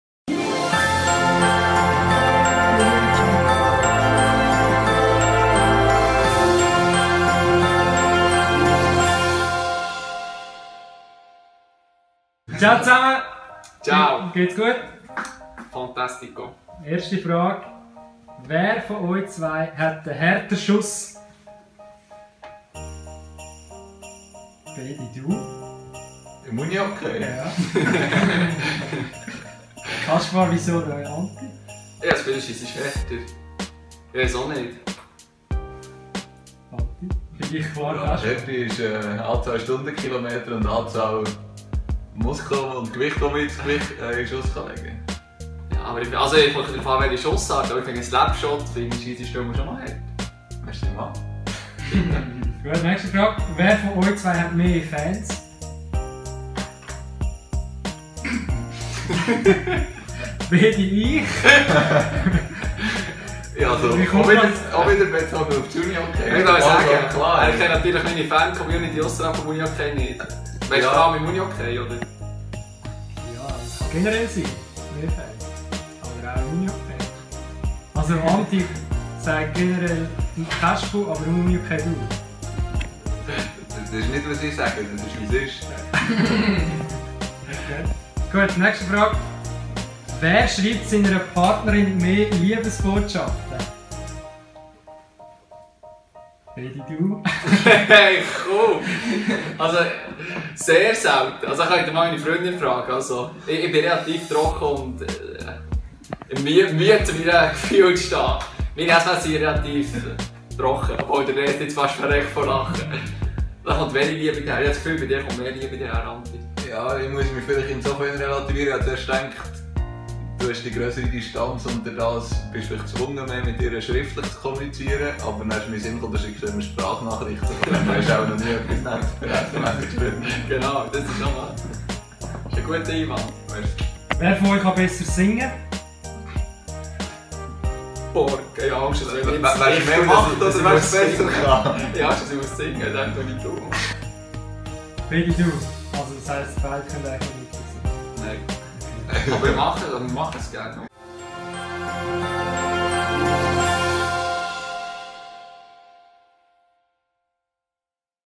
20. Dezember 2015
Emanuel Antener und Kaspar Schmocker kennen sich schon lange. Wie gut kennen sie sich aber wirklich? Unser Harmonietest bringt die Wahrheit ans Licht ;)